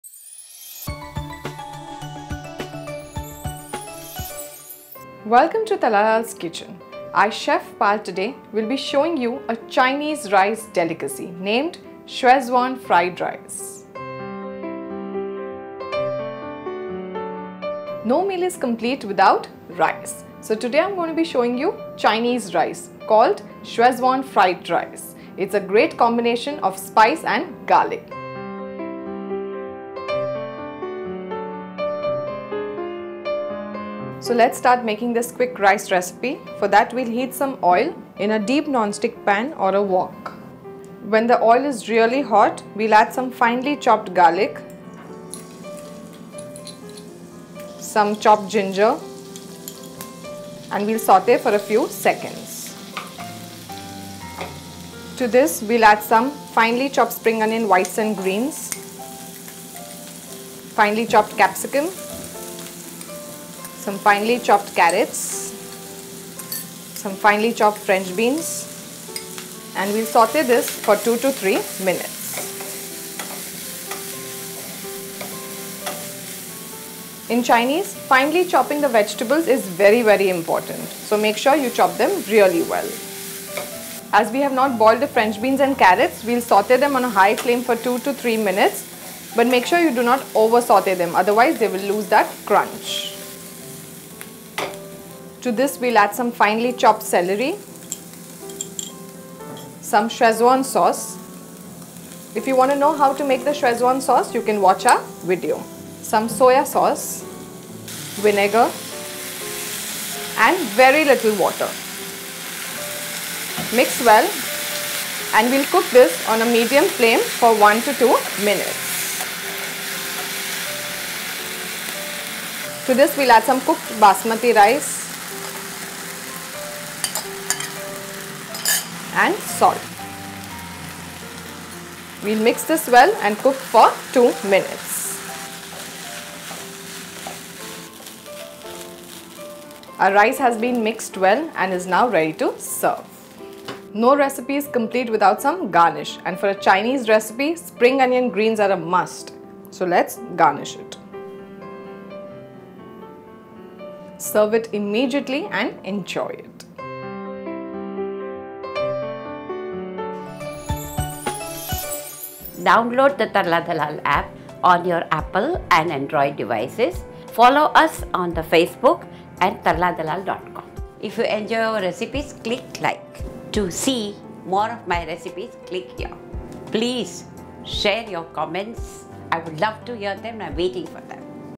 Schezuan Fried Rice by Tarla Dalal
Schezuan Fried Rice, popular Chinese rice made in a jiffy!

Schezuan Fried Rice

Originating from the Sichuan province of China, Schezuan cuisine is characterised by bold flavours, brought about by pungent ingredients like garlic and chillies. Although the original recipe uses special Sichuan peppers that have a lemony taste, in India Schezuan sauce is prepared using red chillies, vinegar and garlic, and it tastes quite authentic. For those who love the lip-smacking fieriness of Schezuan cuisine, here is a quick and easy Schezuan Fried Rice made with long-grained rice, Schezuan sauce, capsicum and other flavourful ingredients.

Serves 4.

6 tbsp Schezuan sauce
3 cups cooked long grained rice (chawal)
1½ tbsp oil
2 tsp finely chopped garlic (lehsun)
1 tsp finely chopped ginger (adrak)
¼ cup finely chopped spring onions (whites and greens)
¼ cup finely chopped capsicum
¼ cup finely chopped carrots
¼ cup finely chopped French Beans
2 tbsp finely chopped celery (ajmoda)
1 tsp soya sauce
1 tsp vinegar

2 tbsp finely chopped spring onion greens

1. Heat the oil in a deep non-stick pan or a wok, add the garlic and ginger and sauté on a medium flame for a few seconds.
2. Add the spring onion whites and greens, capsicum, carrots and French beans and sauté on a high flame for 2 to 3 minutes.
3. Add the celery, Schezuan sauce, soya sauce, vinegar and 2 tbsp of water, mix well and cook on a medium flame for 1 to 2 minutes, while stirring occasionally.
4. Add the rice and salt, mix well and cook on a medium flame for 2 minutes, while stirring occasionally.
    Serve immediately garnished with spring onion greens.